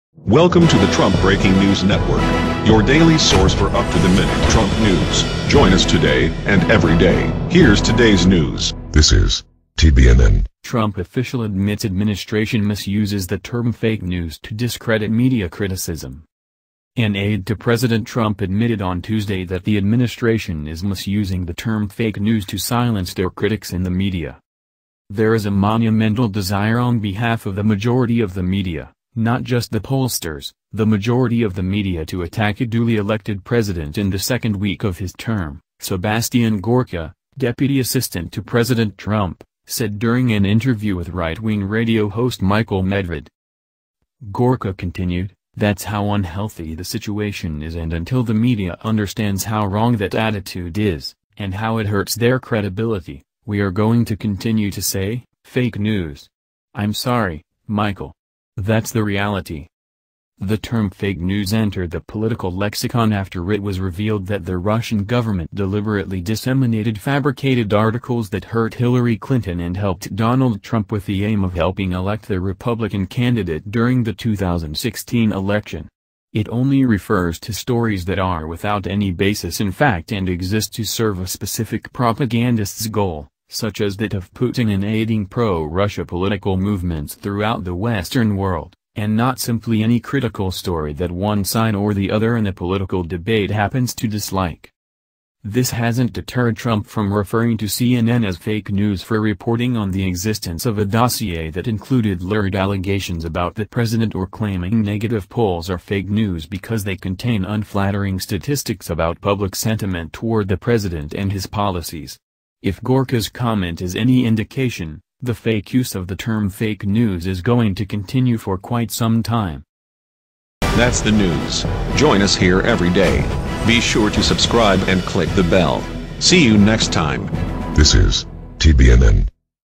Trump official admits administration misuses the term “fake news” to discredit media criticism
Trump Breaking News Network -Trump official admits administration misuses the term “fake news” to discredit media criticism.

An aide to President Trump admitted on Tuesday that the administration is misusing the term “fake news” to silence their critics in the media.

“There is a monumental desire on behalf of the majority of the media, not just the pollsters, the majority of the media to attack a duly elected President in the second week of his term,” Sebastian Gorka, deputy assistant to President Trump, said during an interview with right-wing radio host Michael Medved.

Gorka continued, “That’s how unhealthy the situation is and until the media understands how wrong that attitude is, and how it hurts their credibility, we are going to continue to say, ‘fake news.’ I’m sorry, Michael. That’s the reality.”

The term “fake news” entered the political lexicon after it was revealed that the Russian government deliberately disseminated fabricated articles that hurt Hillary Clinton and helped Donald Trump with the aim of helping elect the Republican candidate during the 2016 election. It only refers to stories that are without any basis in fact and exist to serve a specific propagandist’s goal — such as that of Putin in aiding pro-Russia political movements throughout the Western world — and not simply any critical story that one side or the other in a political debate happens to dislike.

This hasn’t deterred Trump from referring to CNN as “fake news” for reporting on the existence of a dossier that included lurid allegations about the president or claiming negative polls are “fake news” because they contain unflattering statistics about public sentiment toward the president and his policies. If Gorka’s comment is any indication, the fake use of the term “fake news” is going to continue for quite some time.